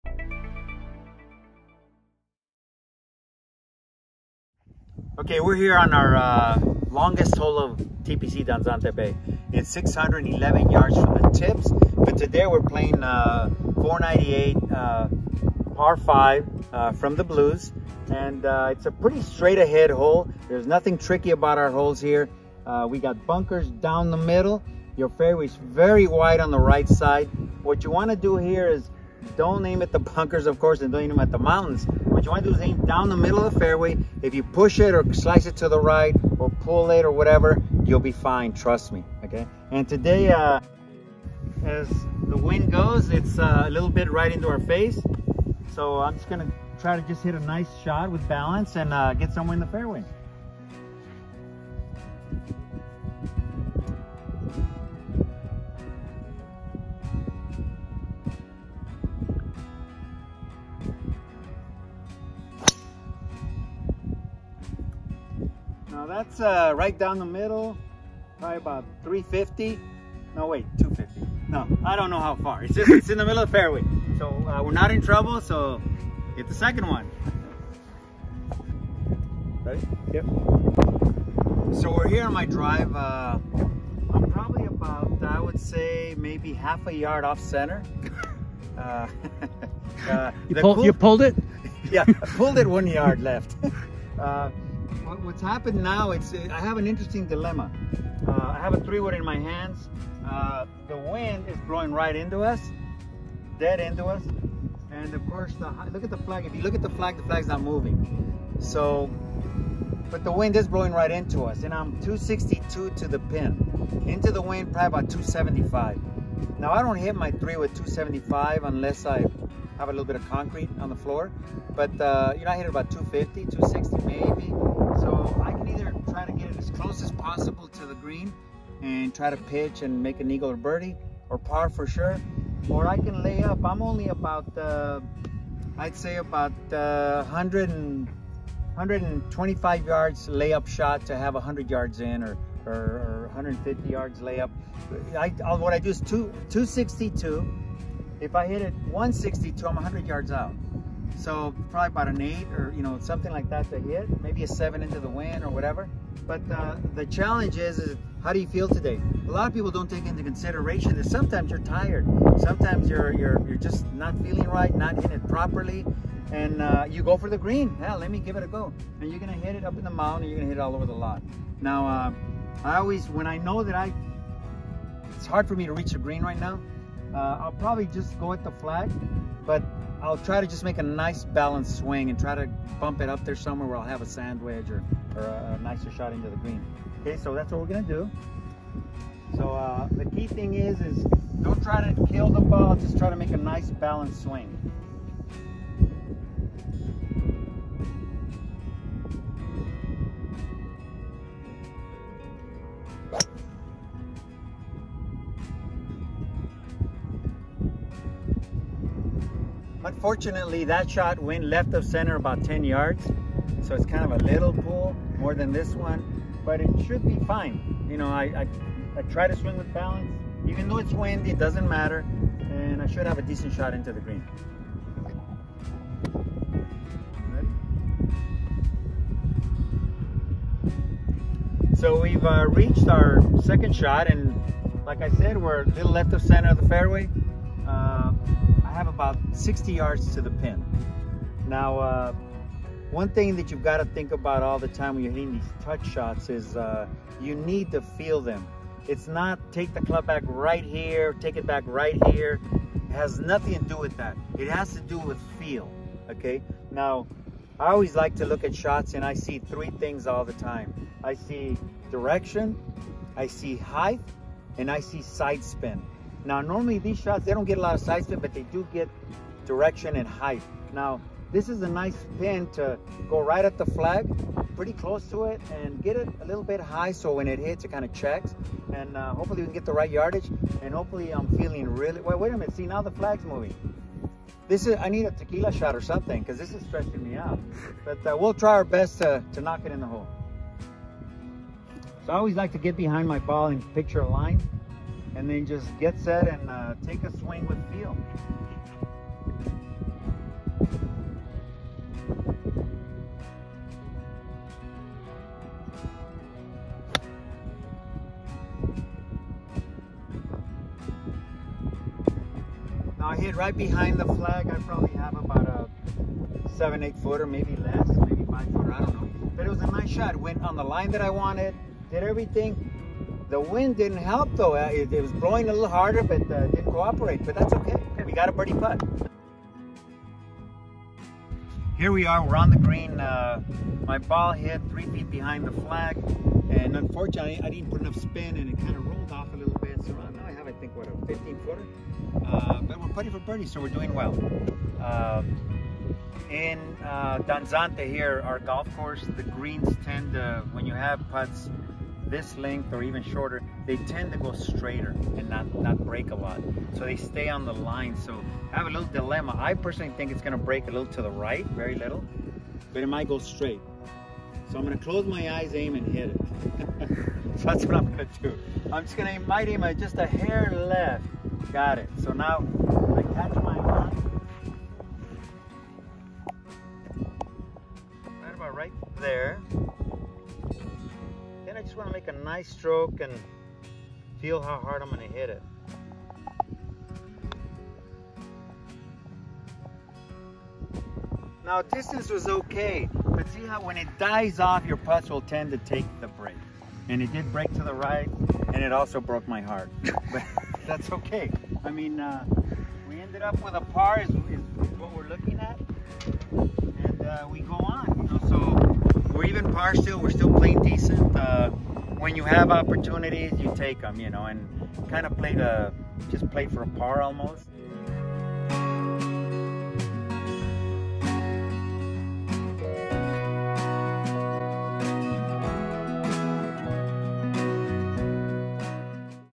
Play the 5th hole at TPC Danzante Bay with Danny Garcia, head pro.
The 5th is the longest hole at TPC Danzante Bay and Danny Garcia takes us through the best way to manage it. Golf at Danzante is a fun and spectacular experience no matter your level of play!